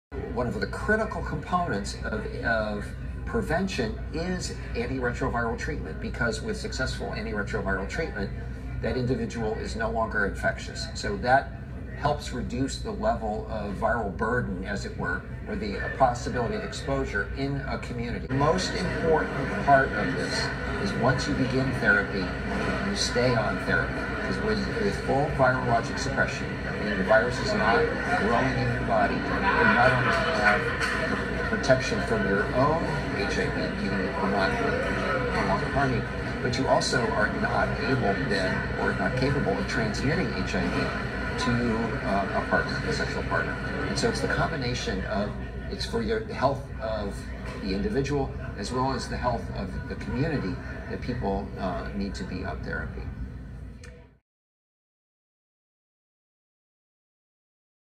8/26/16: Dr. Carl Dieffenbach NIH "Not Infectious"
August 26, 2016: Dr. Carl Dieffenbach, Director of the Division of AIDS, NIH

“The most important part of this is once you begin therapy, you stay on therapy, because with full virologic suppression you not only have protection from your own HIV…but you also are not capable of transmitting HIV to a sexual partner.”

“With successful antiretroviral treatment, that individual is no longer infectious.”

Original Source: blog.aids.gov/2016/08/nihs-dr-carl-dieffenbach-discusses-hiv-treatment-science-at-2016-ryan-white-conference.html